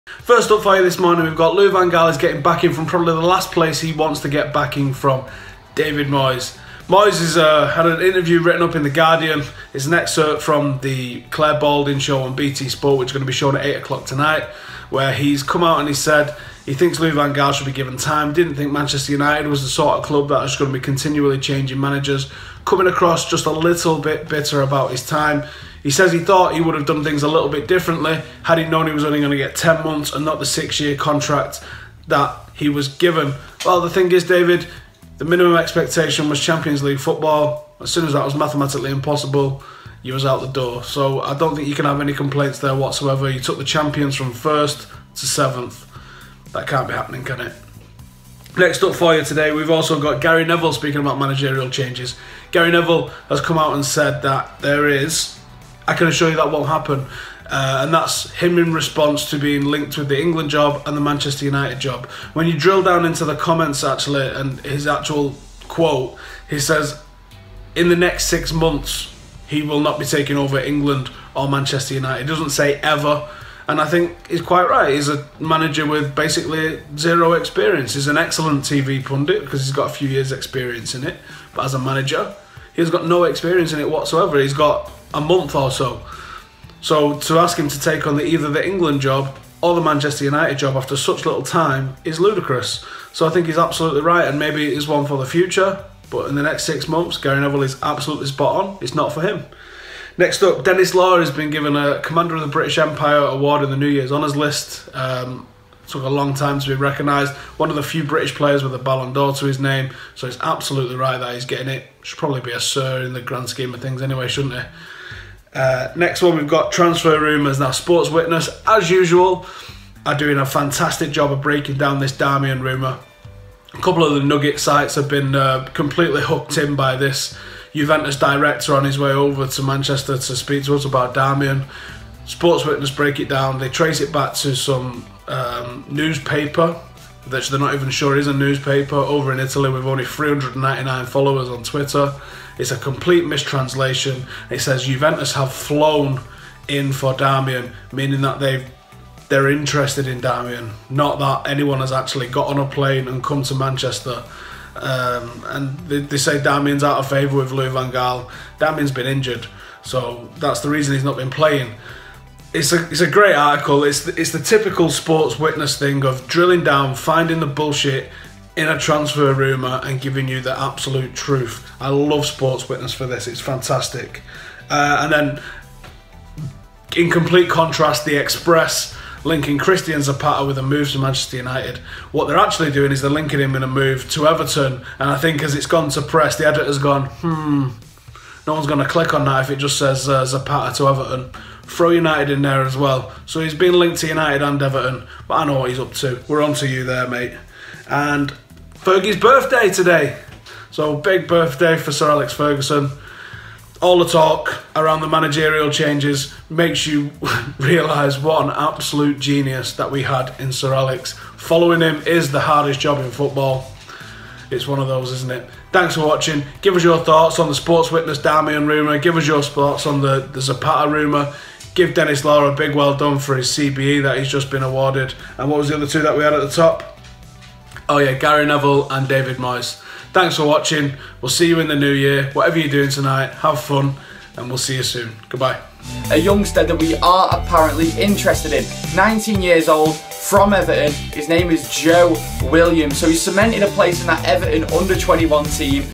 David Moyes: 'He Deserves More Time' | MUFC Daily | Manchester United
Moyes weighs in on the Louis Van Gaal debate; Denis Law given a CBE, Gary Neville on his managerial future and more! MUFC Daily! Your bite-sized Manchester United update, bringing you club and fan community news every weekday! Get your views in below!